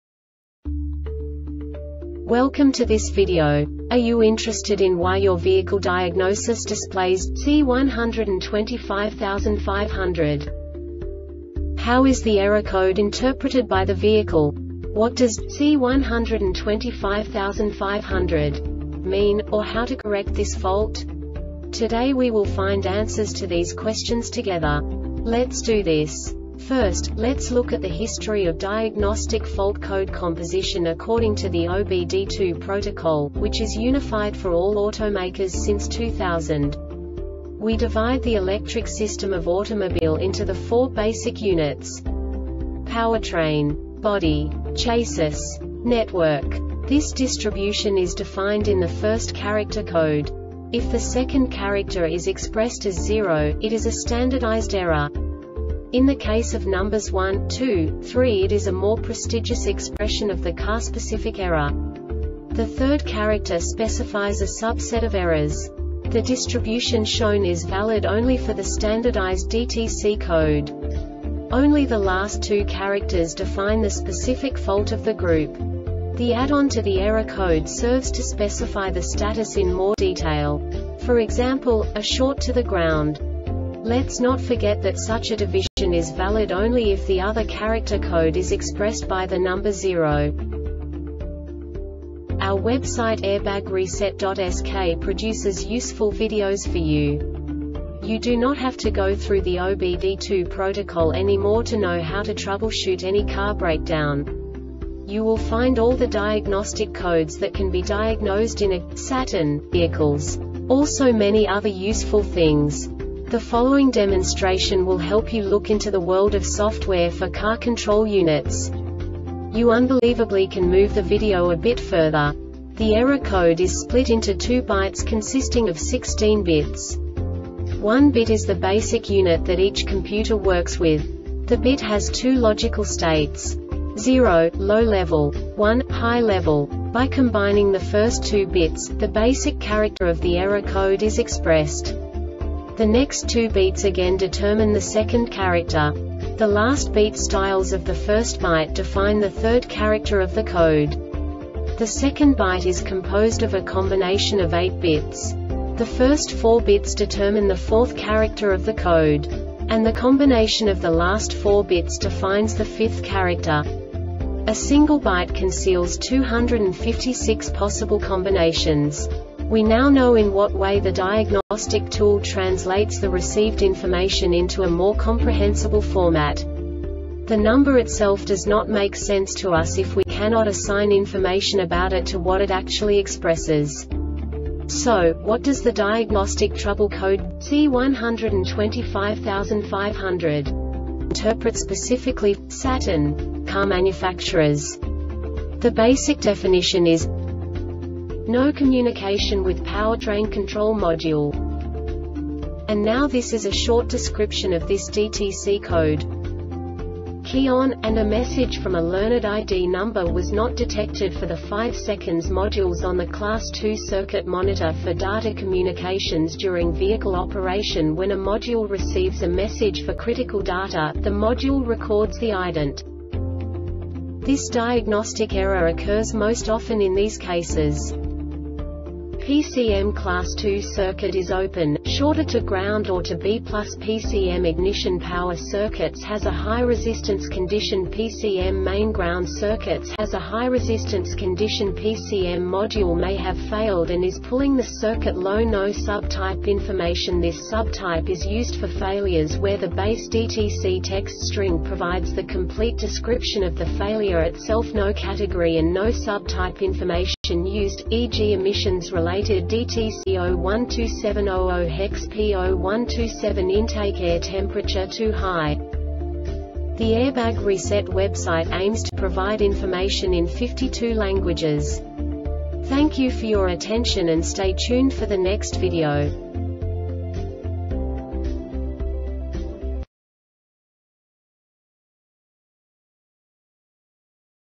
DTC Saturn C1255-00 Short Explanation
Contents:
0:21 Basic DTC analysis according to OBD2 protocol standard.
1:48 Insight into programming
2:58 A diagnostically understandable description of the Diagnostic Trouble Code
3:05 Basic error definition

Make:
Saturn
Code:
C1255 00
Definition:
No Communication With Powertrain Control Module
Description:
Cause:
PCM Class 2 circuit is open,shorted to ground or to B+PCM ignition power circuit(s) has a high resistance conditionPCM main ground circuit(s) has a high resistance conditionPCM (module) may have failed and is pulling the circuit low
Failure Type: